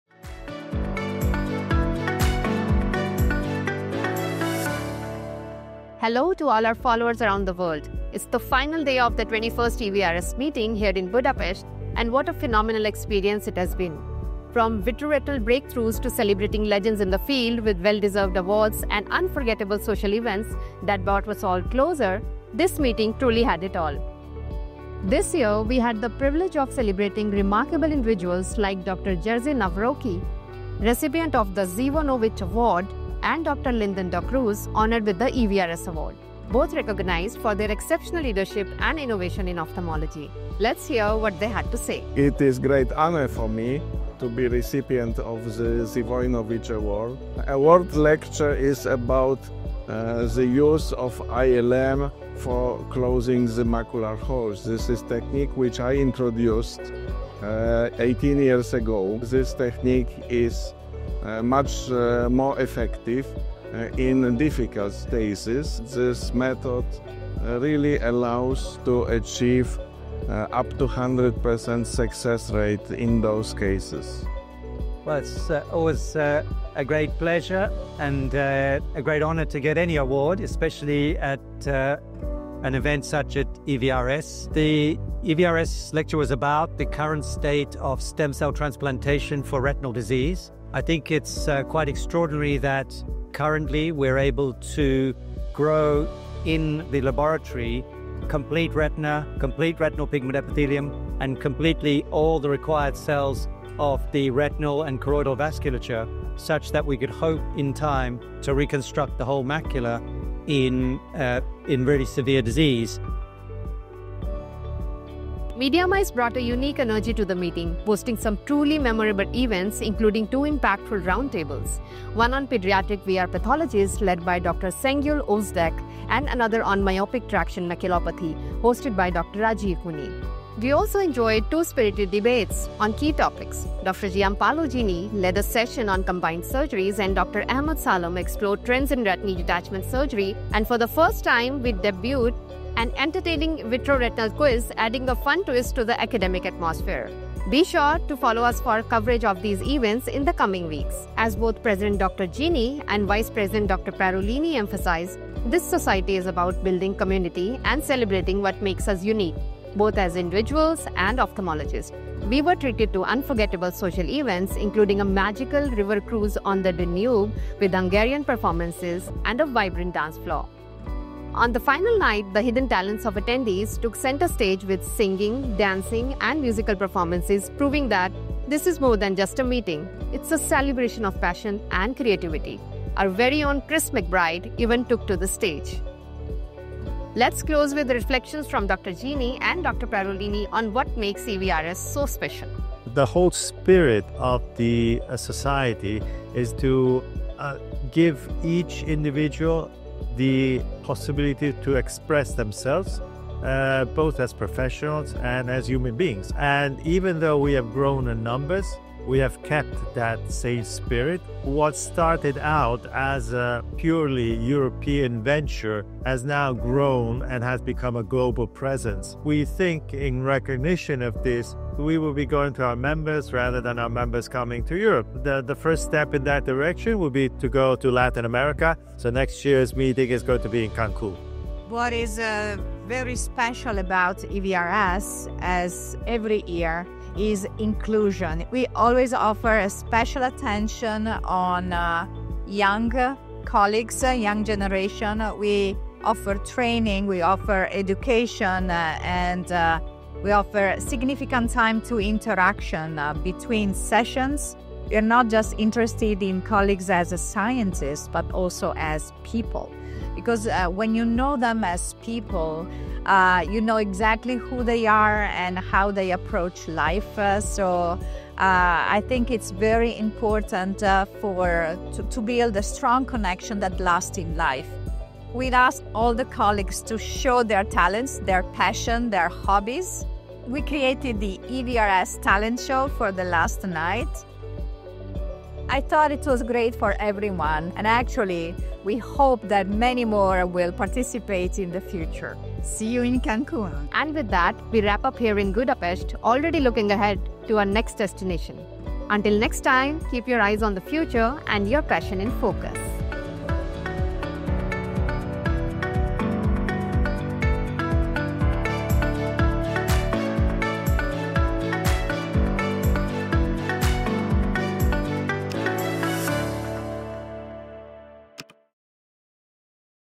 Inside EVRS 2024: Day 4 Highlights
From retina insights to legendary celebrations across the eye care community, Day 4 at EVRS 2024 was nothing short of magical. We’re talking:

-Dr. Jerzy Nawroki dazzling us with his ILM macular hole technique.
-Dr. Lyndon Da Cruz blowing minds with stem cell advancements that could rebuild the retina.
-Spirited debates? Check.
-Pediatric VR pathologies? Covered.
-And yes, we even squeezed in a vitreo-retinal quiz—because learning can be fun!

By nightfall, the Danube River Cruise took center stage with Hungarian performances, dancing, and (brace yourself) a talent show featuring everything from singing to…our very own Chris McBride?

Next stop for EVRS: Cancun 2025! Until then, keep your passion in focus and your eyes on the future. And that’s a wrap!